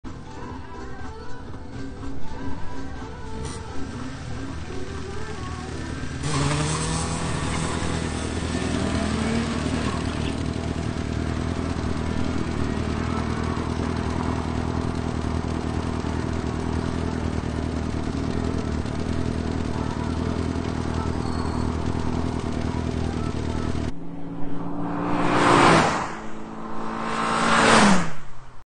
Lexus IS-F vs. Honda civic Drag Race 1/4M (402m), Kámen u Pacova, 27.5.2017, automotosprint.cz
Lexus IS-F vs. Honda civic Drag Race 1/4M (402m)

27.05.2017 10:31

#694 Lexus IS-F
Kategorie: s4
čas 1/4m: 13.405s
rychlost 1/4m: 176.27km/h

Kategorie: s0
čas 1/4m: 15.852s
rychlost 1/4m: 136.53km/h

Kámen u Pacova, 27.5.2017